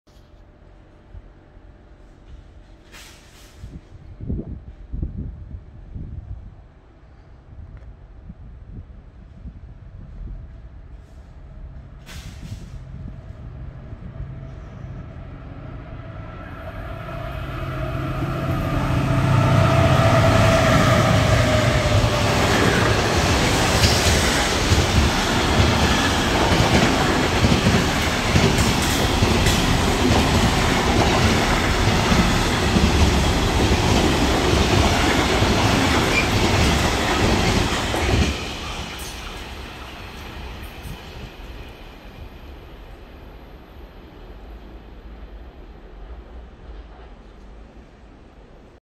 Stadler Eurodual 6000 HHPI 20-02 (90 80 2159 202-1-D-RCM mit Holzzug durch Meschede am 30.01.2021
Am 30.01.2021 um 19:28 Uhr kam die Stadler Eurodual 6000 HHPI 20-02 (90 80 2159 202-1-D-RCM mit einem Holzzug durch den Bahnhof in Meschede. Der Zug kam von der Holzverladung in Arnsberg & fährt nach Österreich.